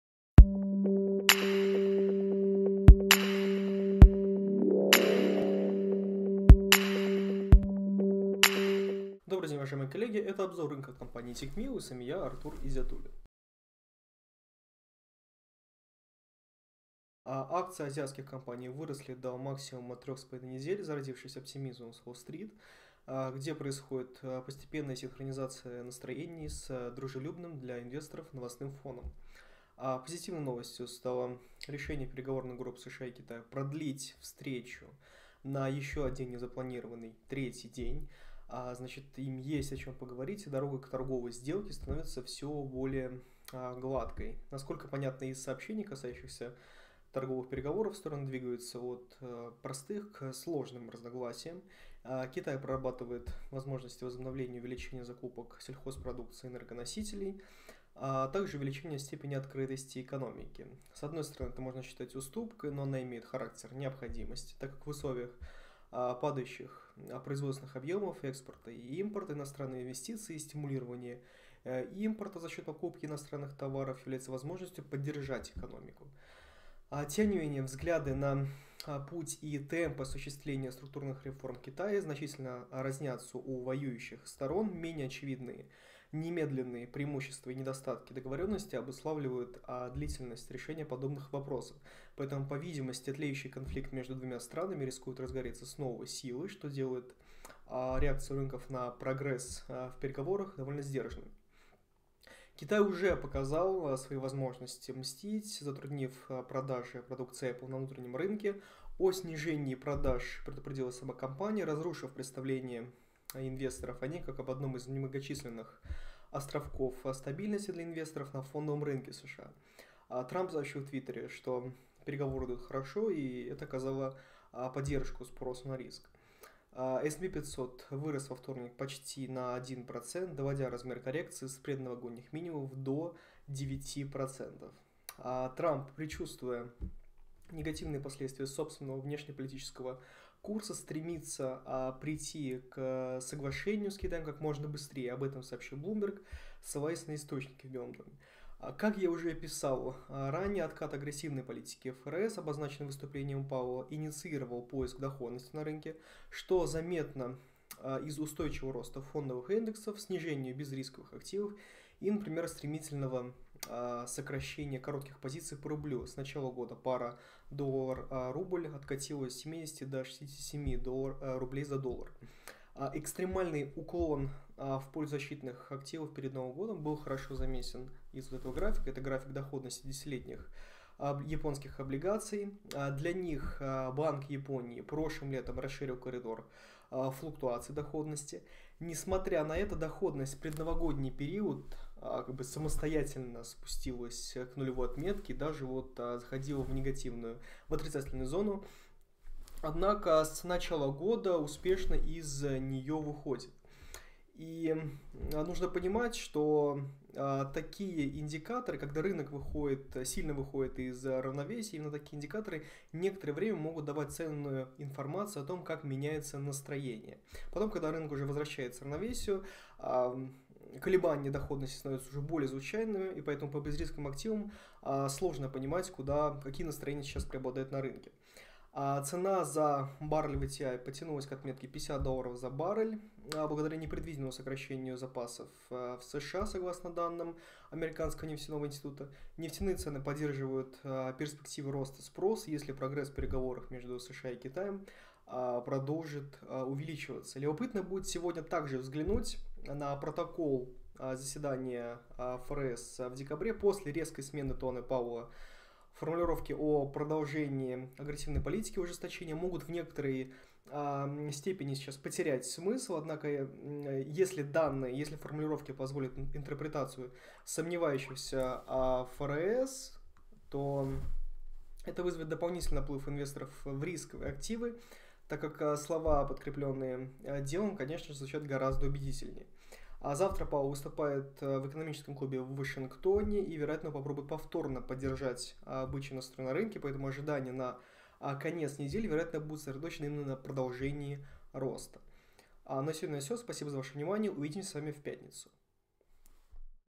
Рынки цепляются за новости о прогрессе в переговорах США и Китая
Больше о Тикмилл:

4. Наш путь к успеху